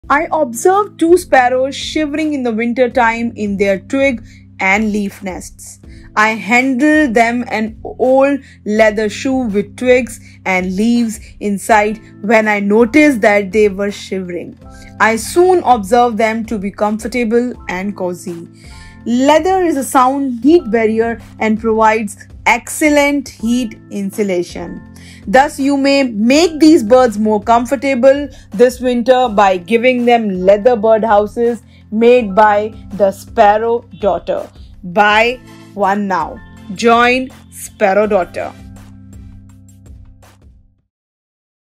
shorts observation
In comparison to other seasons, the sparrow population declines in the winter.❄️

Why?

Because of the cold weather and snow🌨️

During winters the nests made up of twigs and leaves get damaged by the dews and sometimes by heavy snows.

The majority of natural nests are open, which causes the animals to shiver and feel cold.

Therefore, the sparrows will feel more comfortable if leather birdhouses are provided in place of nests.

Leather is a sound heat barrier and provides excellent heat insulation.

You can get one at a much more reasonable price from sparrow daughter

Thus, you may make these birds more comfortable this winter by giving them leather birdhouses made by the sparrow daughter.☺️

Buy one now👈